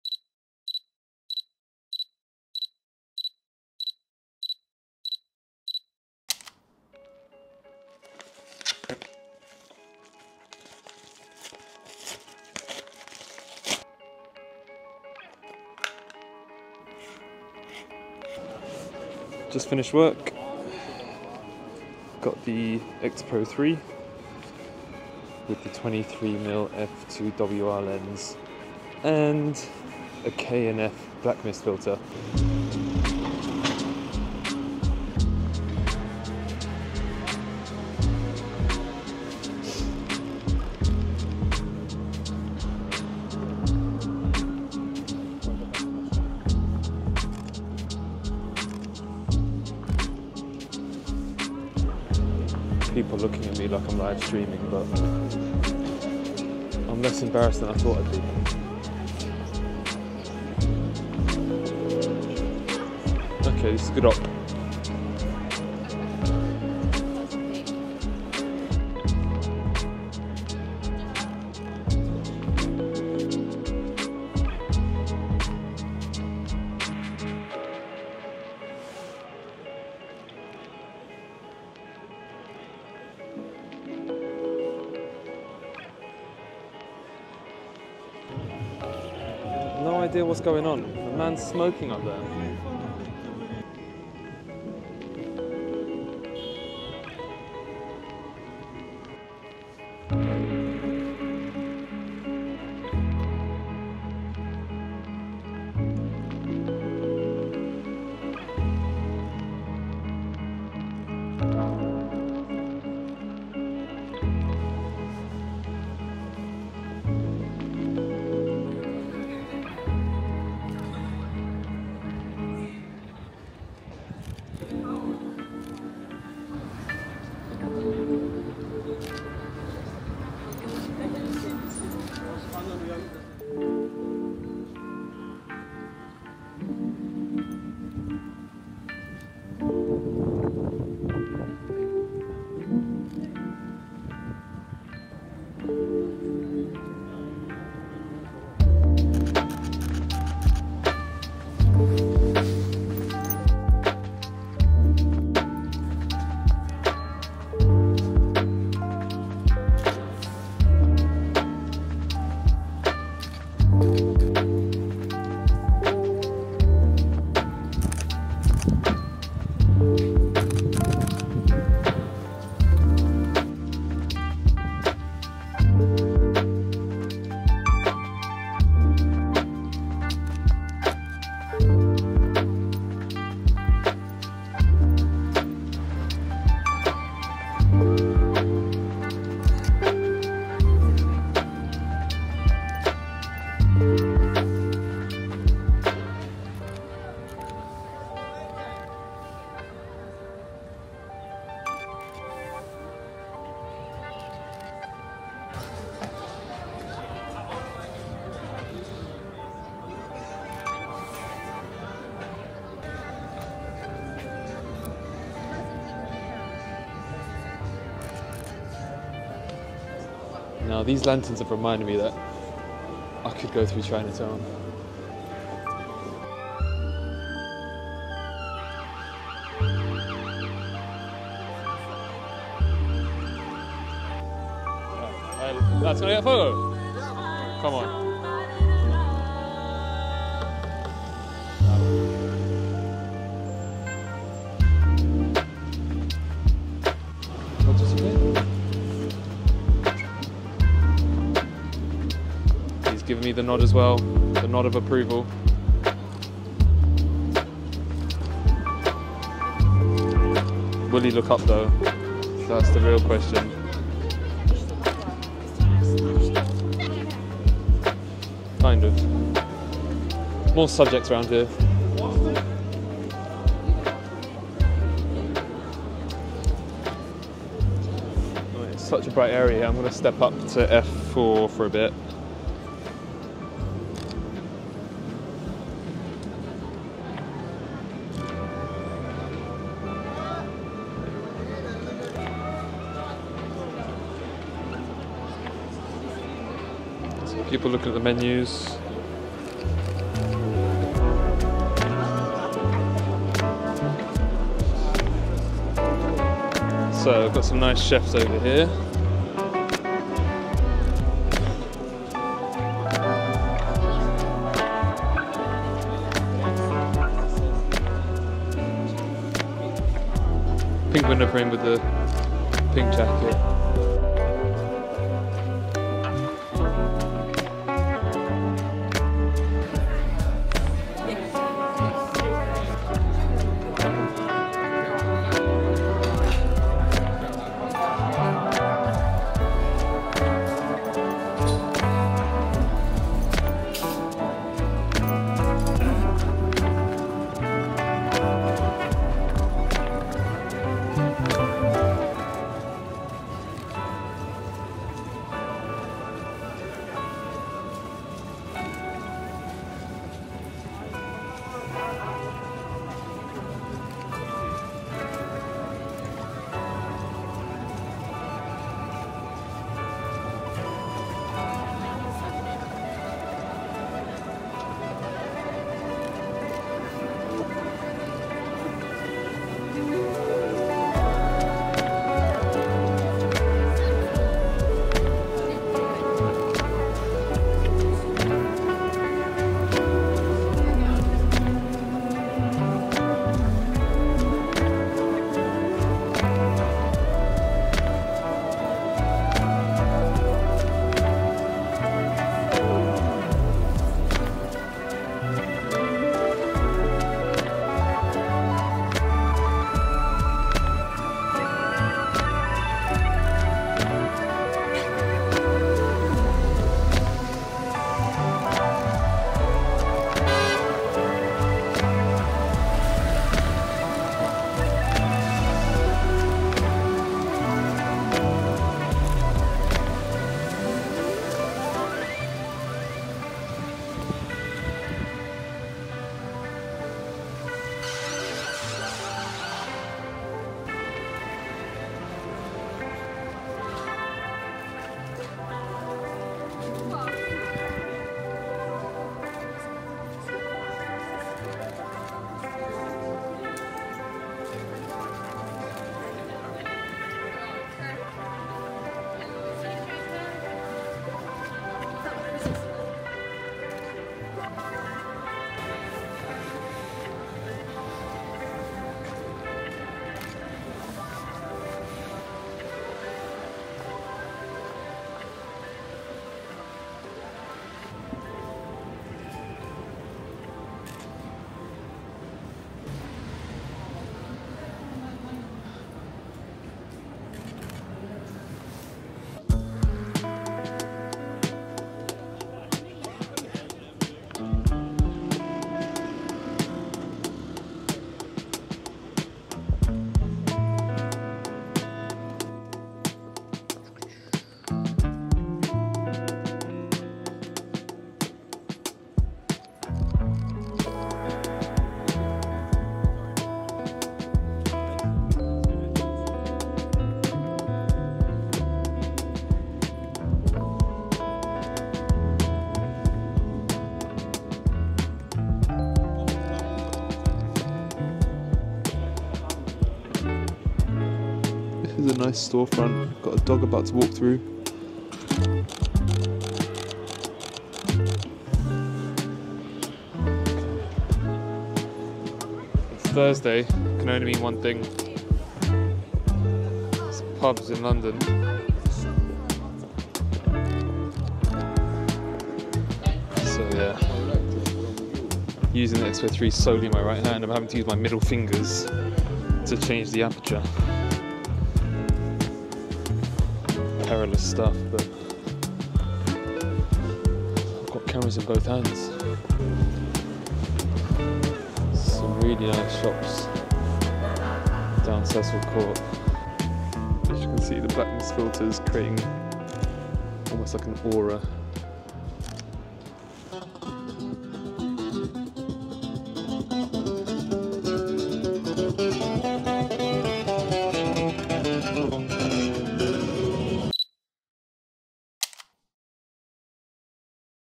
Street Photography at Night | Fujifilm X-Pro3
I rarely do street photography at night. In this POV episode, I'm using a black mist filter on the 23mm f/2, while I commute home from work in central London. I've experimented with the format a bit, using the DJI Osmo Pocket 3 instead of the GoPro (low-light struggles), and adding some live commentary.

Gear Used:

- Fujifilm X-Pro 3
- Fujifilm 23mm f/2
- DJI Osmo Pocket 3 + DJI Mic 2
- Peak Design Cuff Strap

Chapters:

00:00 Introduction
00:39 Regents Street to Trafalgar Square
02:13 The Southbank
03:38 Chinatown
06:43 Rainy Soho
08:09 Commute to Charing Cross